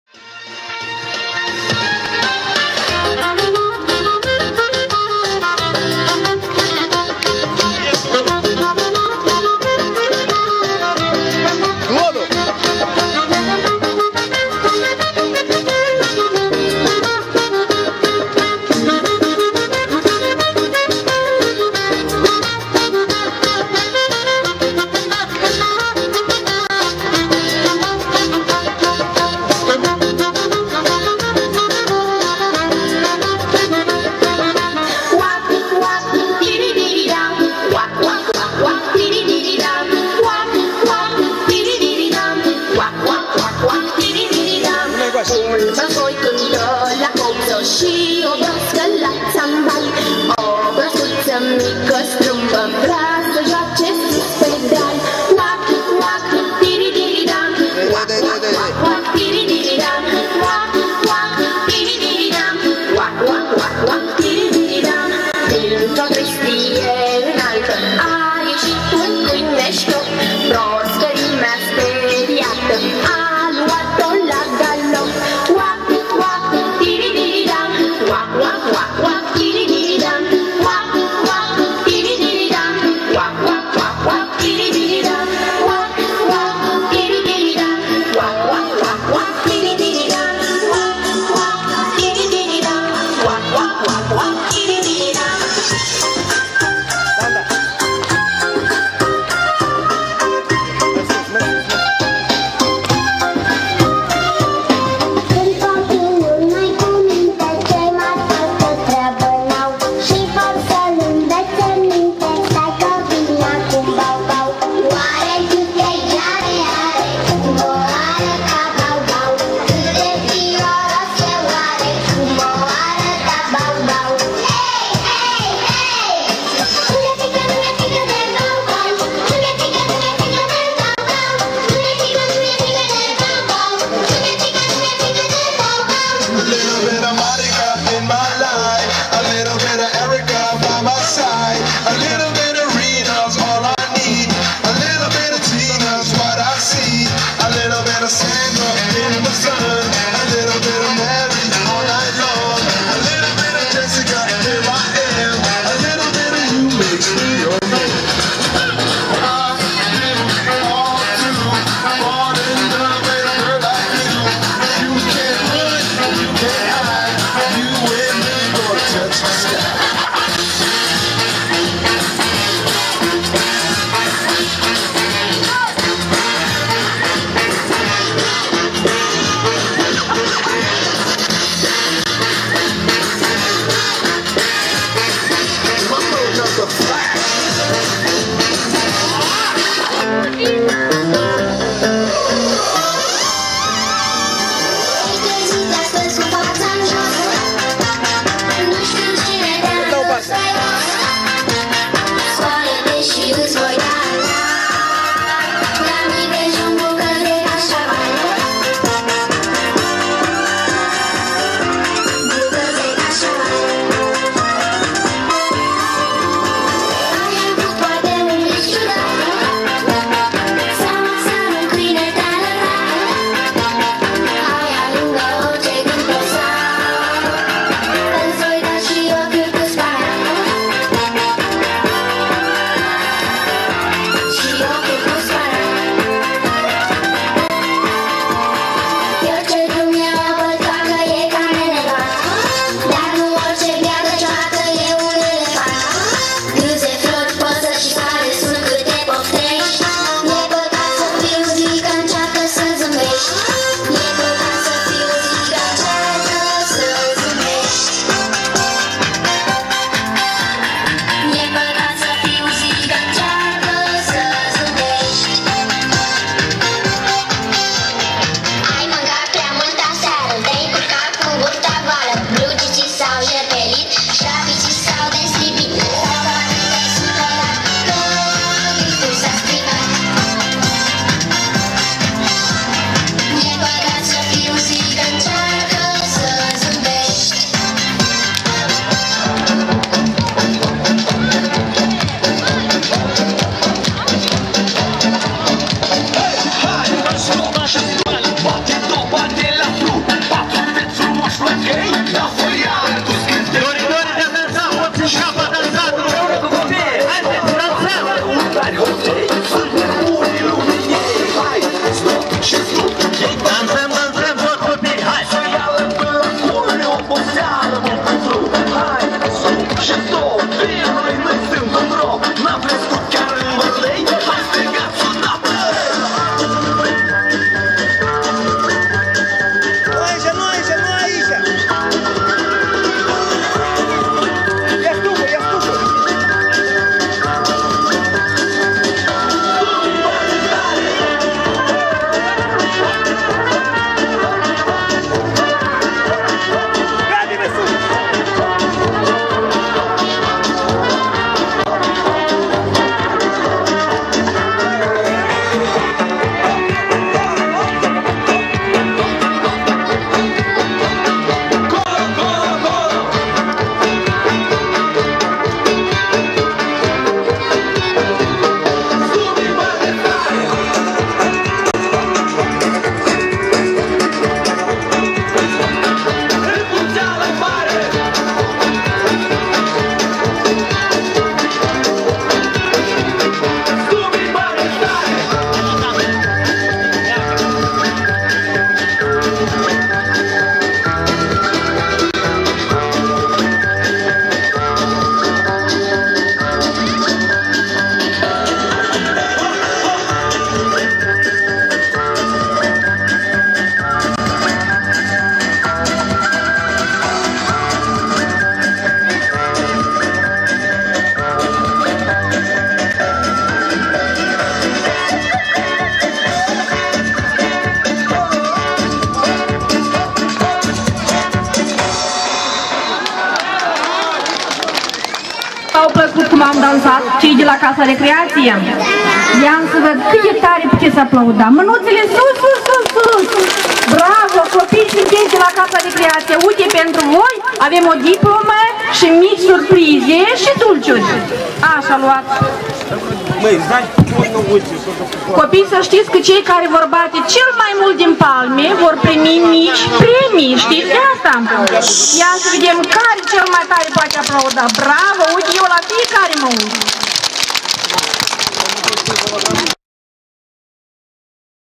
1 IUNIE 2012 EUROPA CIMISLIA
Ansamblul EUROPA danseaza in centrul orasului  CIMISLIA,  1 IUNIE 2012 ZIUA COPILULUI.